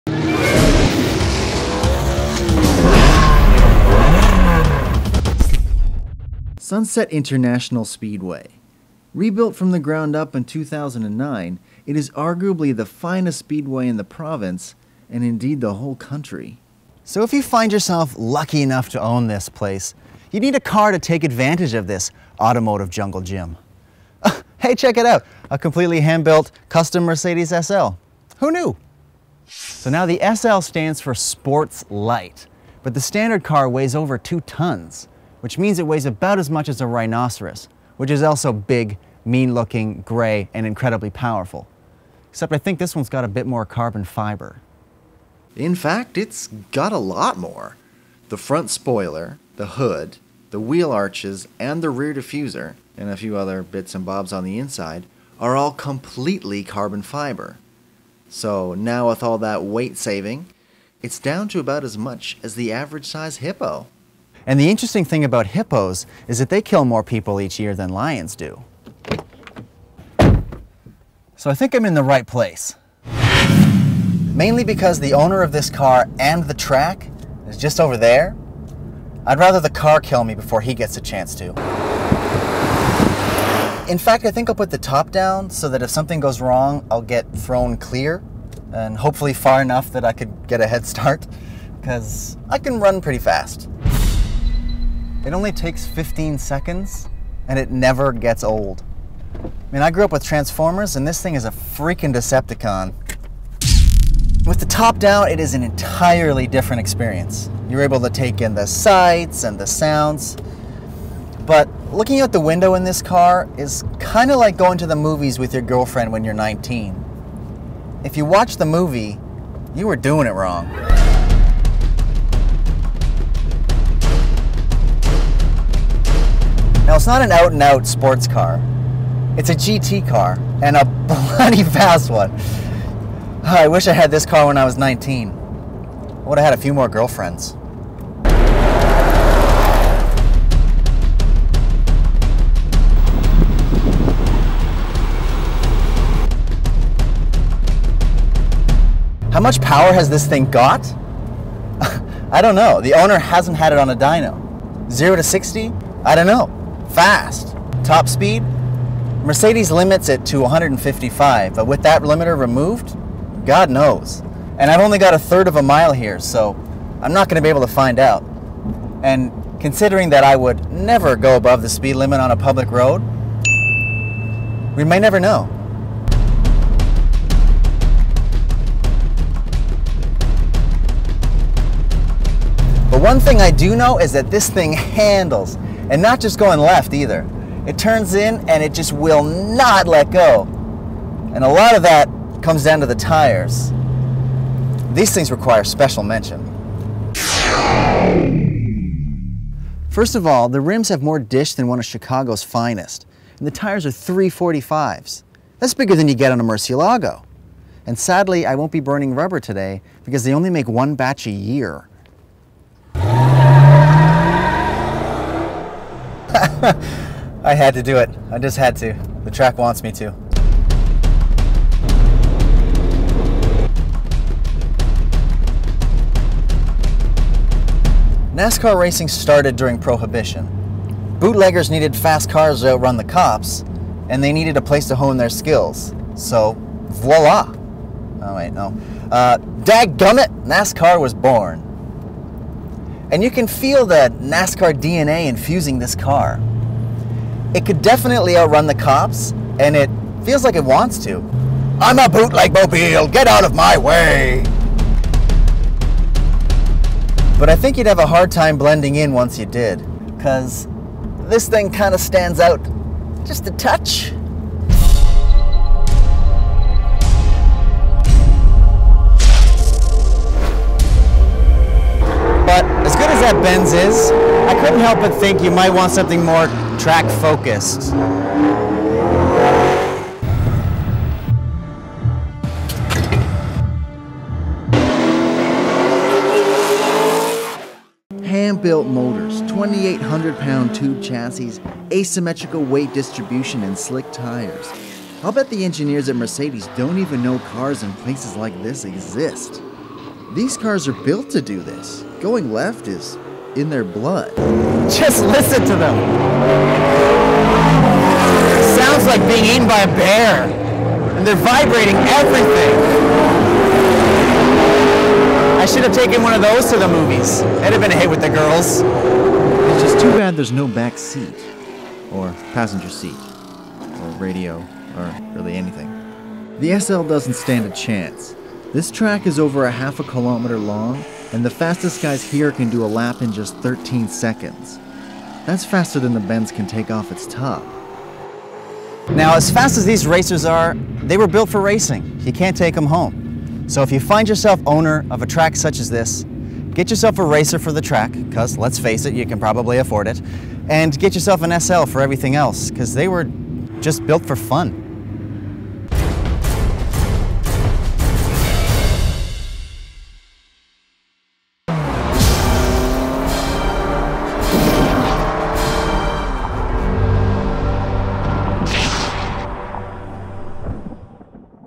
Mercedes SL custom widebody Tracktest REVIEW
OVERSTEER.TV Review of a 2011 Custom widebody Mercedes SL.

On location at the beautifully renovated Sunset Speedway, Ian Thomson track tests one of the hottest custom Mercedes SLs in Canada to find out how it stacks up against insanely fast custom built Super Stock race cars.